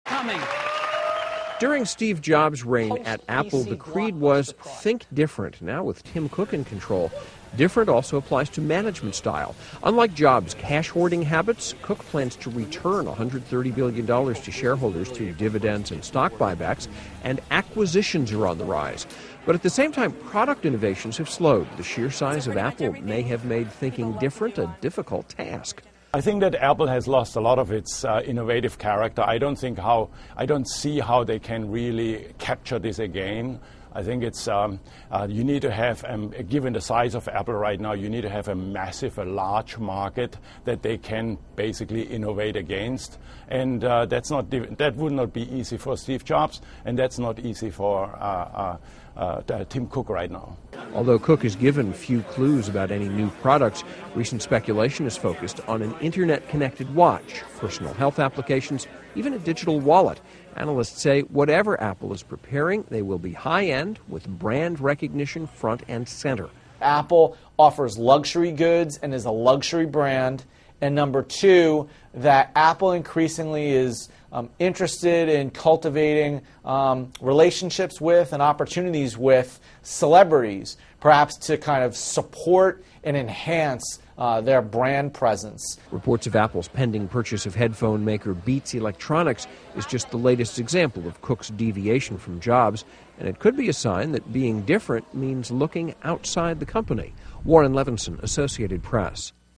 Apple CEO Dares to Be Different From Steve Jobs
"Think different" became Apple's creed during the late Steve Jobs' reign as CEO. Now, chief executive Tim Cook is embracing the idea while making decisions that would have seemed crazy to his fabled predecessor. (May 12)